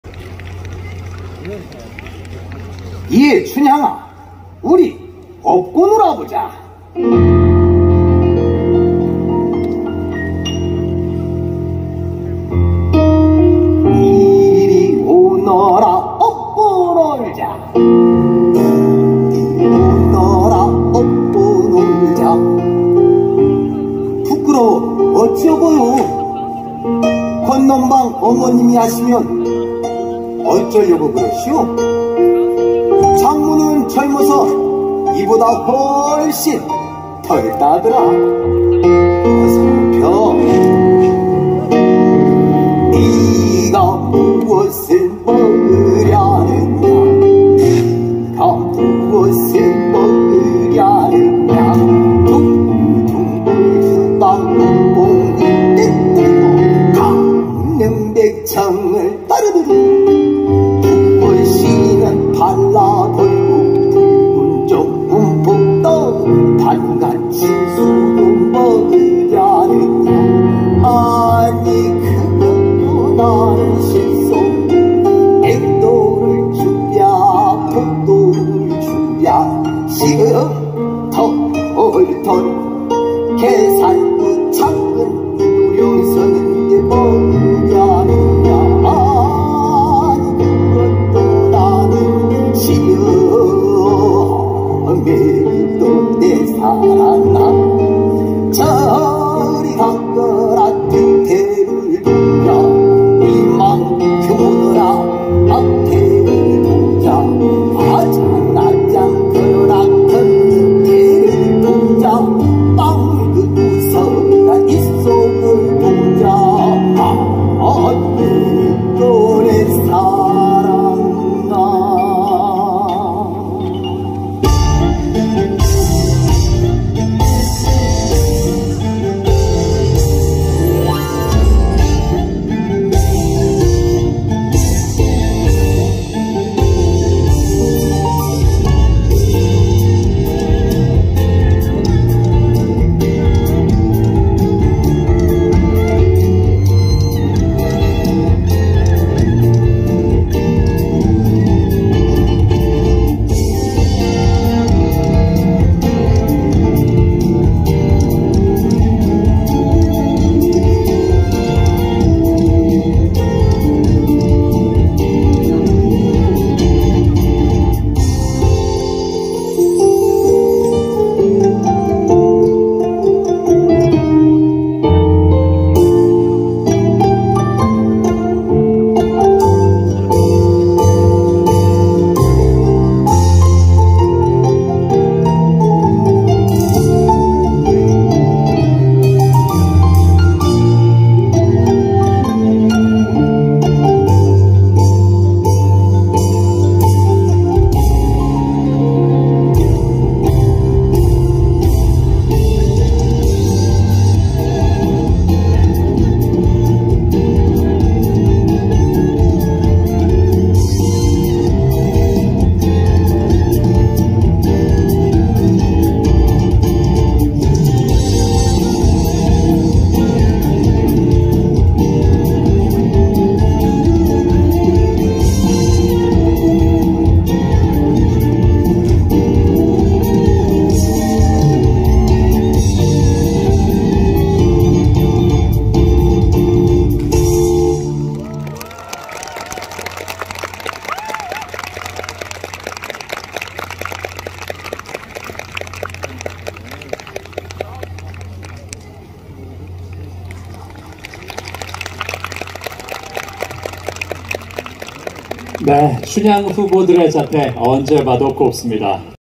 제92회 전국춘향선발대회 한국무용|춘향후보 8번 이송현|2022.5.4
2022 제92회 미스춘향선발대회 한국무용 영상 입니다🎥
춘향후보 8번 19세 이송현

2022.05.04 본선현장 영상